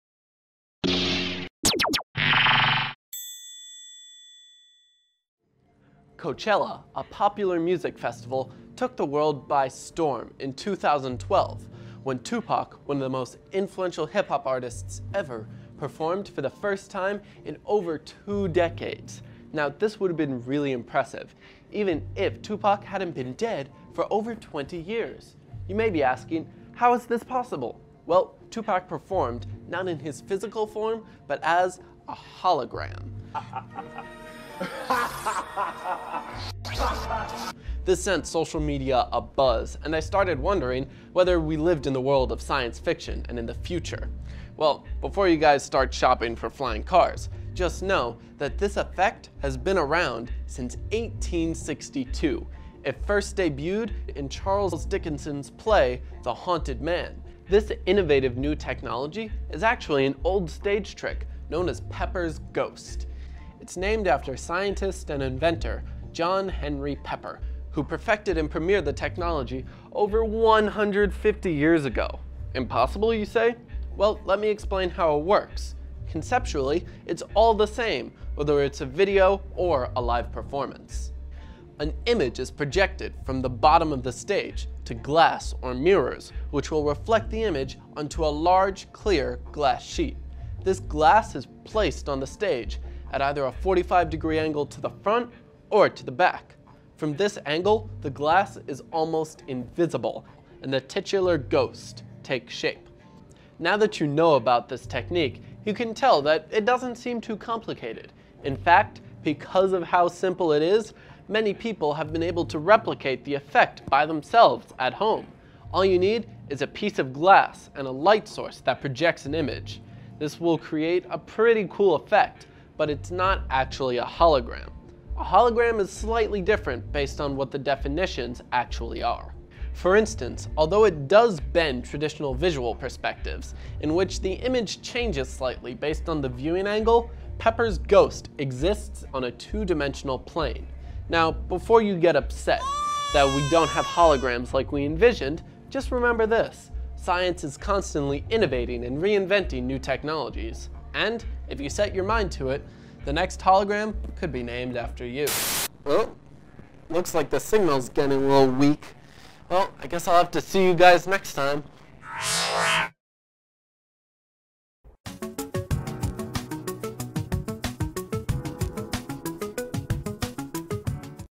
Performing Holograms – The Past or Future?
Iconic performers (e.g: Michael Jackson and Tupac) brought back to life with the futuristic technology of holograms, a visual spectacle once only seen in science fiction films, now apparently realized. Yet this “new” technology isn't exactly so new, in fact, it’s first form came in a performance on Christmas day nearly 200 years ago. An old stage trick called Pepper's ghost, with some modern tweaking still has a mind-bending effect on today's audiences. The video will explain what Pepper's ghosts is and why it is definitely not a hologram.

Coachella- a popular music festival- took the world by storm in 2012, when it brought Tupac, one of the biggest stars in hip hop, back to the stage after over a decade for a spectacular performance.

Now, that alone would have been captivating if Tupac wasn't already dead for over 15 years. You may be asking, “How is this possible?”  Well, Tupac performed not in physical form, but as a “hologram”. This performance blew up all over social media outlets, shocking the masses who believed we had finally entered the world of science fiction. Before you start shopping for flying cars, you might be interested to know the that first form of this Holographic technology was debuted in Charles Dickens’ play called, The Haunted Man, on Christmas in the year 1862. This innovative new technology is actually an old stage trick called Pepper's Ghost. It’s named after scientist and inventor, John Henry Pepper, who perfected and premiered the technology over 150 years ago. Impossible you say? Well, let me explain how it worked back then through today. Conceptually it's all the same- whether it’s a video or a live performance. The image is projected from the bottom of the stage to mirrors or glass and is then reflected off of them to a large clear glass sheet. This glass is placed at a 45 degree angle from either the front or back of the stage. From this angle, the glass becomes almost invisible and the titular ghost takes shape. So now that you know the technique, it doesn't seem so complicated, right?  In fact, because of the simplicity of this trick, many people have been able to do it by themselves at home. As long as you have a piece of glass and a light source that projects an image, the holograms will appear. So although it is quite a cool effect, it doesn't match our definition of what a hologram is in actuality. For instance, although it bends traditional visual perspectives, in which the image does change slightly based on your viewing angle, Pepper's Ghost exists on a two-dimensional plane. But, before you get upset that we don't have the holograms like we envisioned, just remember this. We are constantly innovating and reinventing new technologies every day, and if you set your mind on it, the next hologram could be named after you!

Black screen.

Presenter (V.O)
Ah, not this guy again.

Clicking sound. End.

Producer, Writer, Sound & Editor: Brandon Palmieri
Director & Camera: Brittany
Talent: David Meer
Special Effects: Khalid Straker